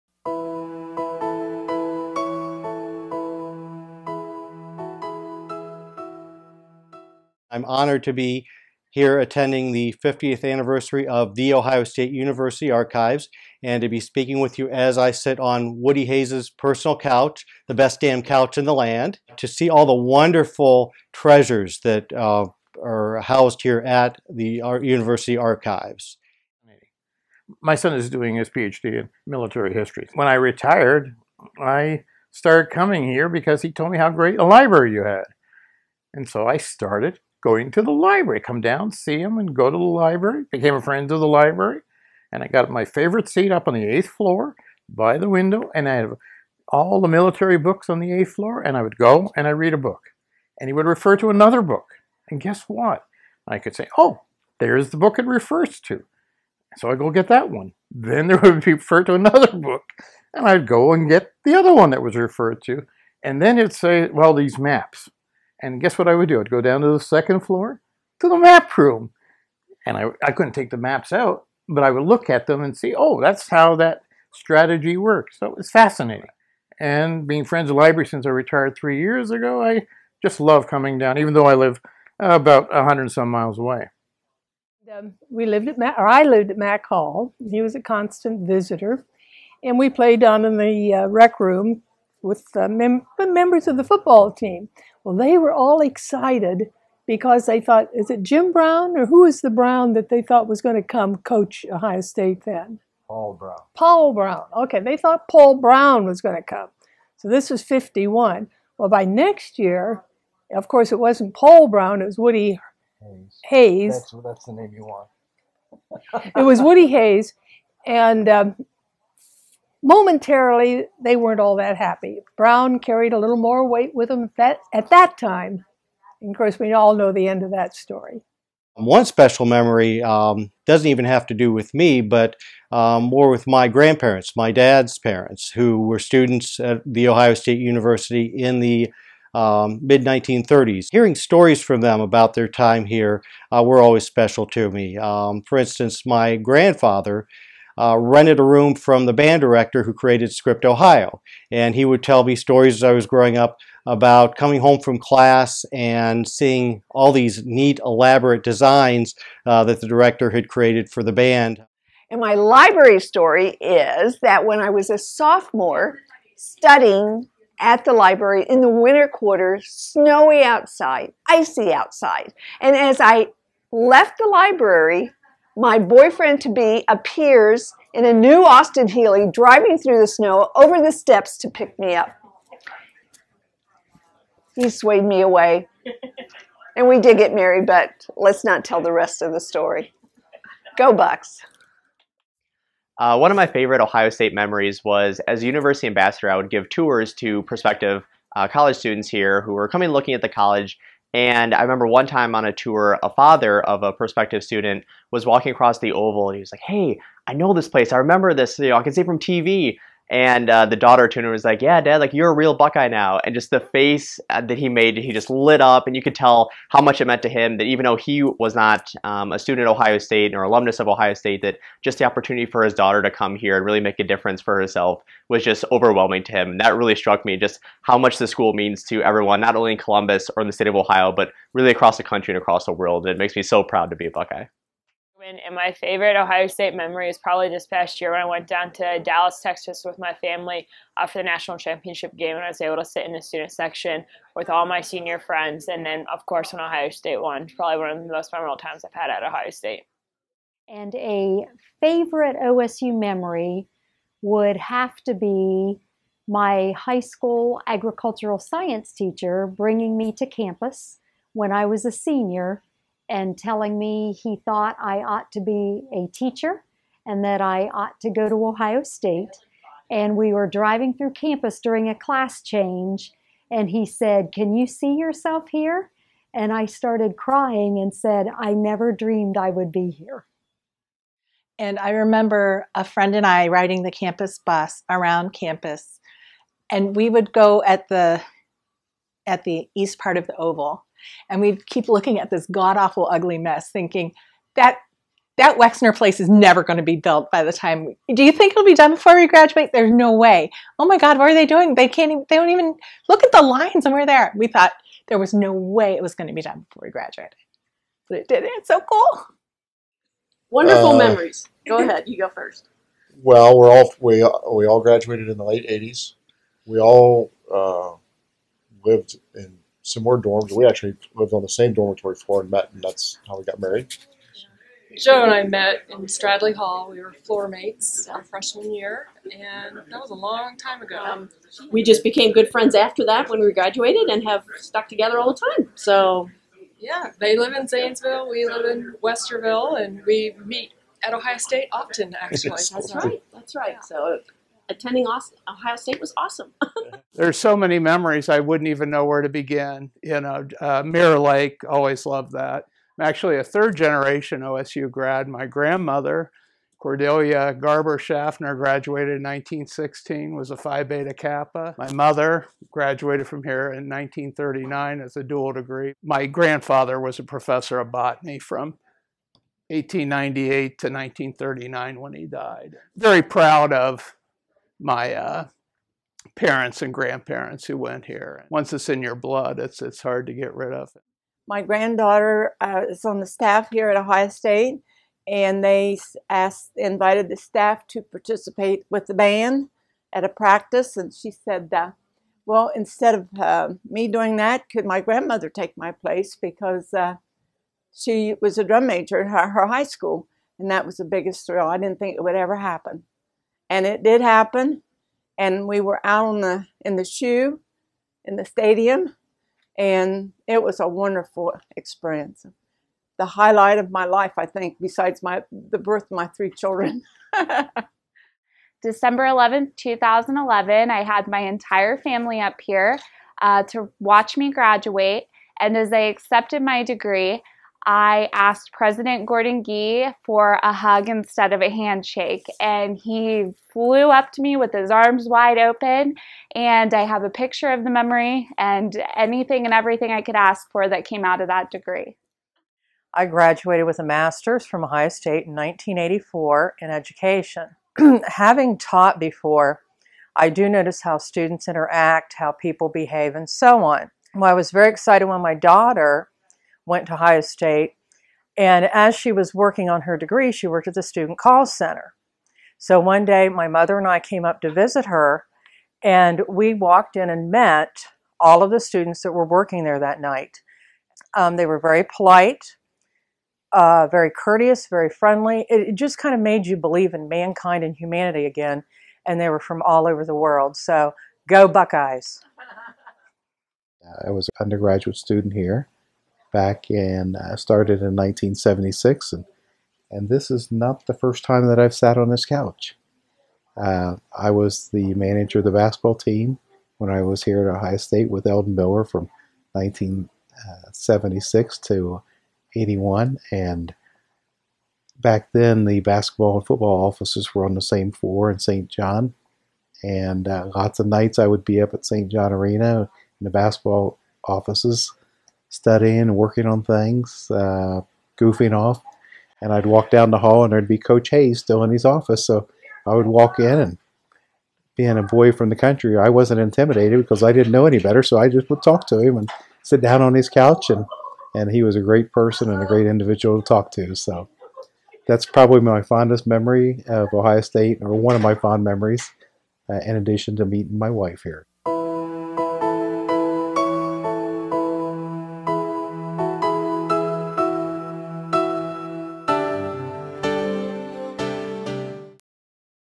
Buckeye Memories
Buckeyes share favorite memories of The Ohio State University during the 50th anniversary celebration of University Archives.